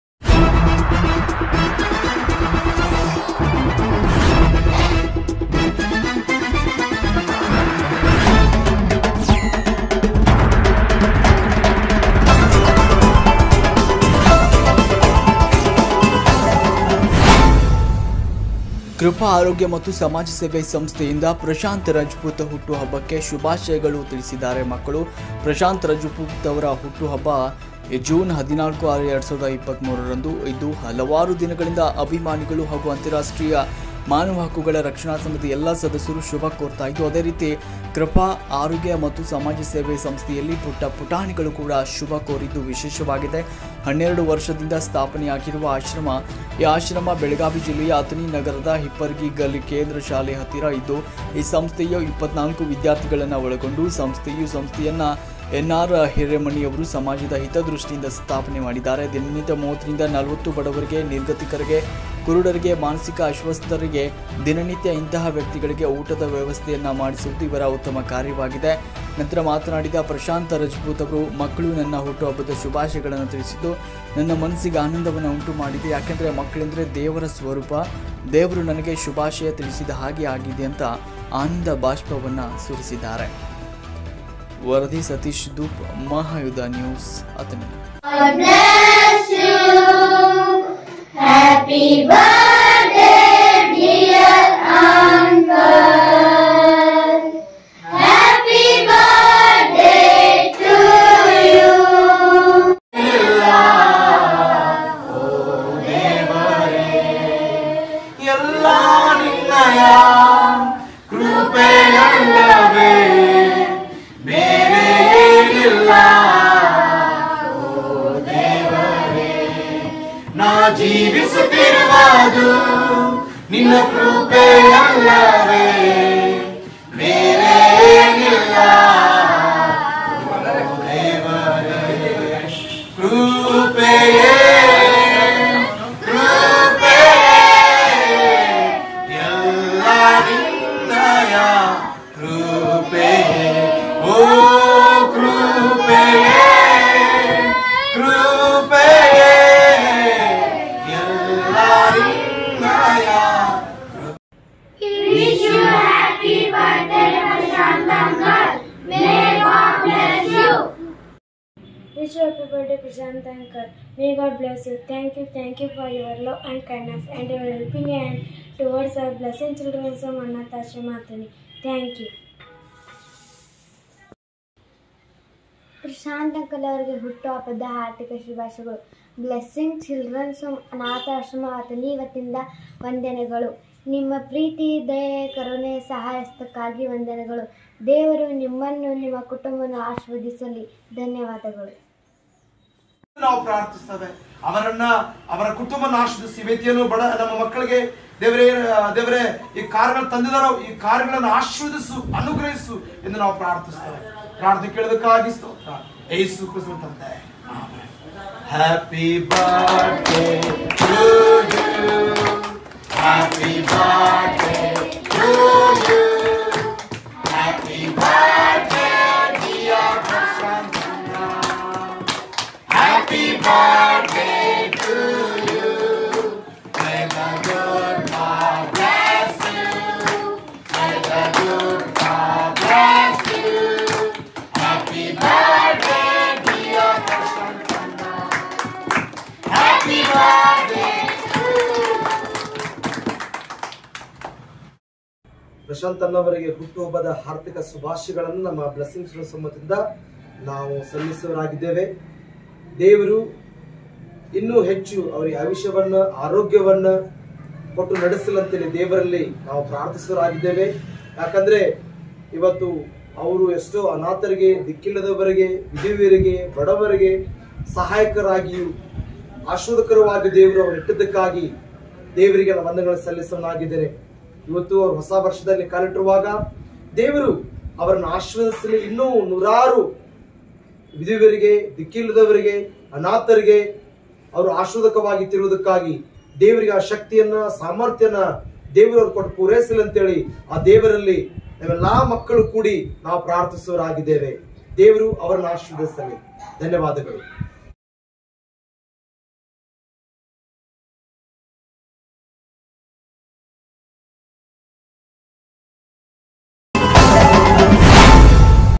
ಕ್ರಪಾ ಆರೋಗ್ಯ ಮತ್ತು ಸಮಾಜ ಸೇವೆ ಸಂಸ್ಥೆಯಿಂದ ಪ್ರಶಾಂತ ರಜಪೂತ ಹುಟ್ಟುಹಬ್ಬಕ್ಕೆ ಶುಭಾಶಯಗಳು ತಿಳಿಸಿದ
ಕ್ರಪಾ ಆರೋಗ್ಯ ಮತ್ತು ಸಮಾಜ ಸೇವೆ ಸಂಸ್ಥೆಯಿಂದ ಪ್ರಶಾಂತ ರಜಪೂತ ಹುಟ್ಟುಹಬ್ಬಕ್ಕೆ ಶುಭಾಶಯಗಳು ತಿಳಿಸಿದ ಮಕ್ಕಳು

ಪ್ರಶಾಂತ ರಜಪುತ ಅವರ ಹುಟ್ಟುಹಬ್ಬವು ಜೂನ 14/06/2023 ರಂದು ಇದ್ದು ಹಲವಾರು ದಿನಗಳಿಂದ ಅಭಿಮಾನಿಗಳು ಹಾಗೂ ಅಂತಾರಾಷ್ಟ್ರೀಯ ಮಾನವ ಹಕ್ಕುಗಳು ರಕ್ಷಣಾ ಸಂಘದ ಎಲ್ಲ ಸದಸ್ಯರು ಶುಭ ಕೋರುತ್ತಿದ್ದು ಅದೇ ರೀತಿ ಕ್ರಪಾ ಆರೋಗ್ಯ ಮತ್ತು ಸಮಾಜ ಸೇವೆ ಸಂಸ್ಥೆಯಲ್ಲಿ ಪುಟ್ಟ ಪುಟಾಣಿಗಳು ಶುಭ ಕೊರಿದ್ದು ವಿಶೇಷವಾಗಿದೆ

12 ವರ್ಷದಿಂದ ಸ್ಥಾಪನೆಯಾದ ಆಶ್ರಮ
ಈ ಆಶ್ರಮವು ಬೆಳಗಾವಿ ಜಿಲ್ಲೆಯ ಅಥಣಿ ನಗರದ ಹಿಪ್ಪರಗಿ ಗಲ್ಲಿ ಕೇಂದ್ರ ಶಾಲೆ ಹತ್ತಿರ ಇದ್ದು ಈ ಸಂಸ್ಥೆಯು 24 ವಿದ್ಯಾರ್ಥಿಗಳನ್ನು ಒಳಗೊಂಡ ಸಂಸ್ಥೆಯು
ಈ ಸಂಸ್ಥೆಯನ್ನು ಎನ ಆರ ಹಿರೇಮಣಿ ಅವರು ಸಮಾಜದ ಹಿತದೃಷ್ಟಿಯಿಂದ ಸ್ಥಾಪನೆ ಮಾಡಿದ್ದಾರೆ
ದಿನನಿತ್ಯ 30 ರಿಂದ 40 ಬಡವರಿಗೆ, ನಿರ್ಗತಿಕರಿಗೆ, ಕುರುಡರಿಗೆ ಮಾನಸಿಕ ಅಶ್ವಸ್ತರಿಗೆ ದಿನನಿತ್ಯ ಇಂತಹ ವ್ಯಕ್ತಿಗಳಿಗೆ ಊಟದ ವ್ಯವಸ್ಥೆ ಮಾಡುಸುವುದು  ಇವರ ಉತ್ತಮ ಕಾರ್ಯವಾಗಿದೆ

ನಂತರ ಮಾತನಾಡಿದ ಪ್ರಶಾಂತ ರಜಪುತ ಮಕ್ಕಳು ನನ್ನ ಹುಟ್ಟು ಹಬ್ಬದ ಶುಭಾಶಯವನ್ನು ತಿಳಿಸಿದ್ದು ನನ್ನ ಮನ್ನಸ್ಸಿಗೆ  ಆನಂದ ಉಂಟು ಮಾಡಿದೆ ಏಕೆಂದರೆ ಮಕ್ಕಳೆಂದರೆ ದೇವರ ಸ್ವರೂಪ ದೇವರ ನನಗೆ ಶುಭಾಶಯ ತಿಳಿಸಿದ ಹಾಗೆ ಆಗಿದೆ ಎಂದು ಆನಂದ ಬಾಷ್ಪವನ್ನು ಸುರಿಸಿದರು

ವರದಿ.. ಸತೀಶ ಧೂಪ ಮಹಾಯುದ್ದ ನ್ಯೂಸ